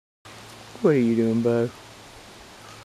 Video of adoptable pet named Bo
Bo is an adoptable pet with Richardson Rescue in York SC.